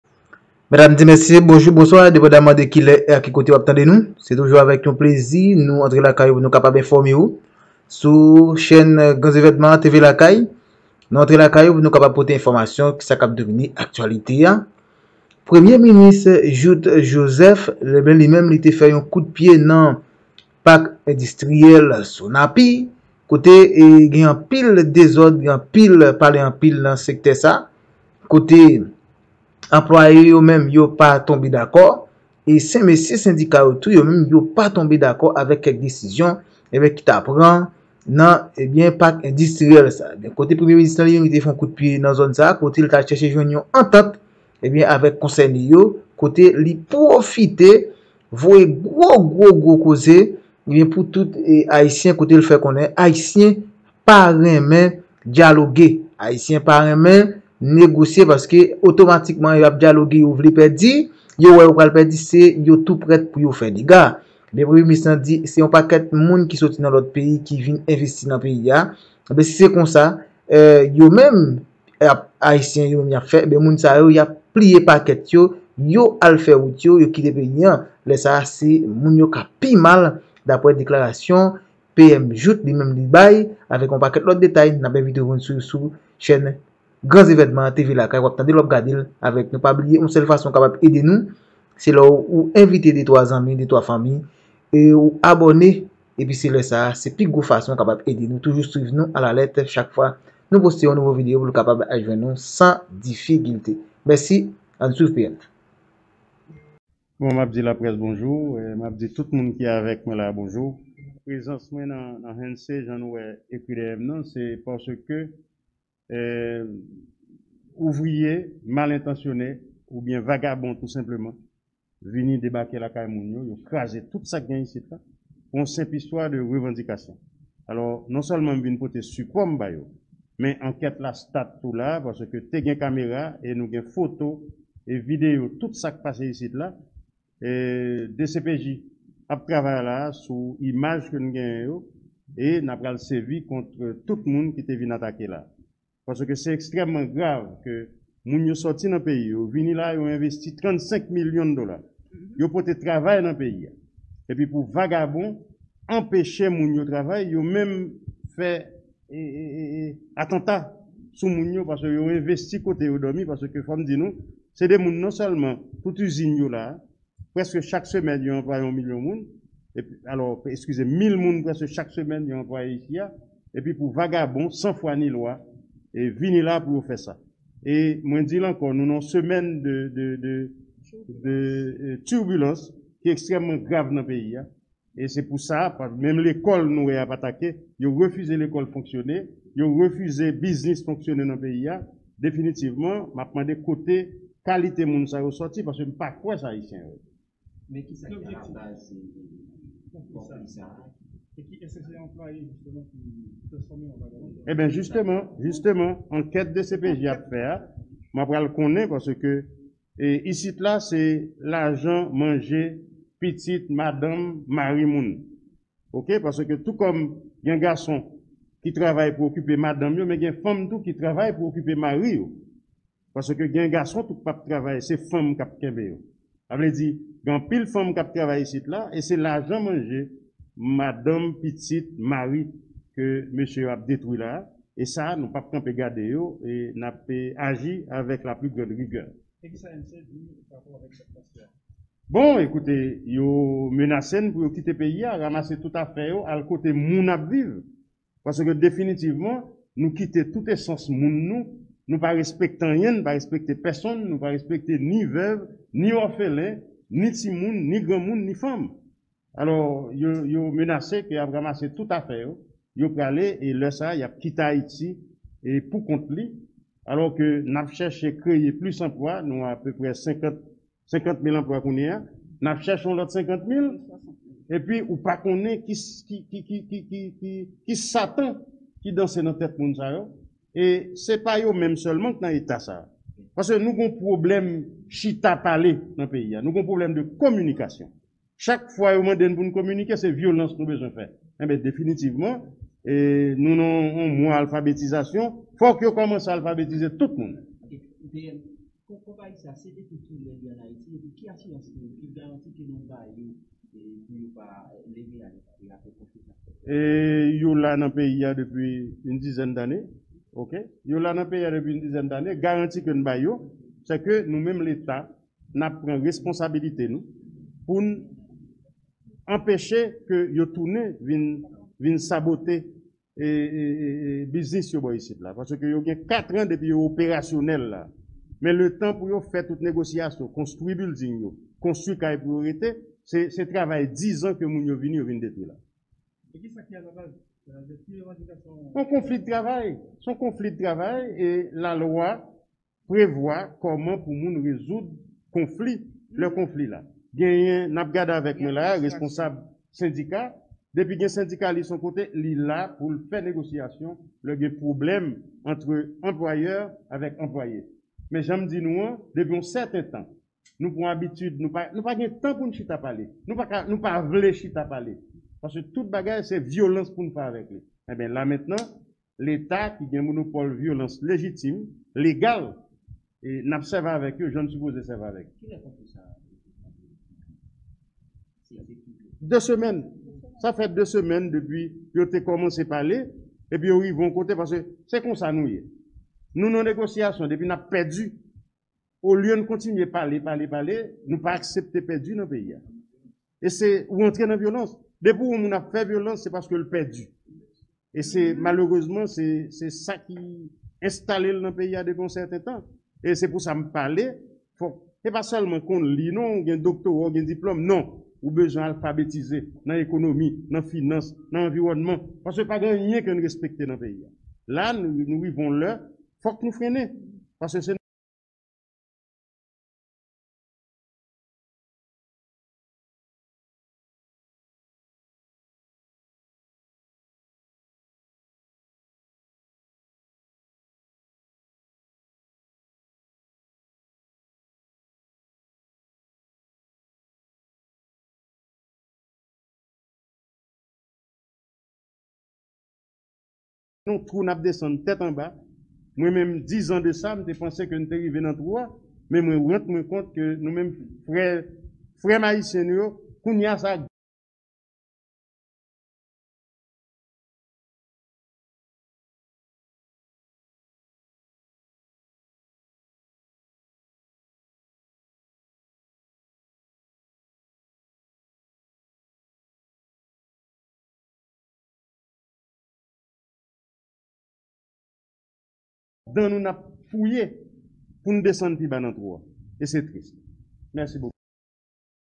PM Jouthe dedouble nan SONAPI li kase met nanmen moun kap kreye latwoublay nan sektè a
Premye Minis Jouthe Joseph fè yon gwo vizit nan SONAPI li kase met nanmen Sindaka yo ki refize dyaloge pou yo ka kreye panik nan peyi a pou plis detay klike la